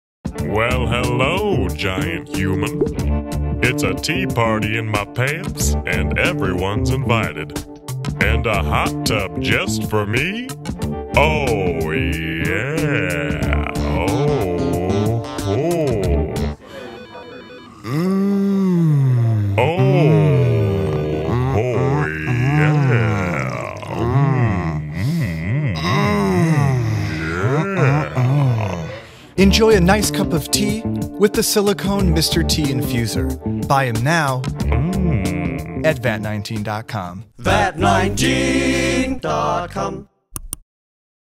Relax with the Mr. Tea Infuser
Brew your loose leaf tea in style with the always-relaxed Mr. Tea Infuser.

Load up his silicone pants with tea, perch him atop your cup, and watch as he effortlessly steeps your tea.

The dishwasher and microwave-safe Mr. Tea Infuser is the perfect gift for any lover of tea.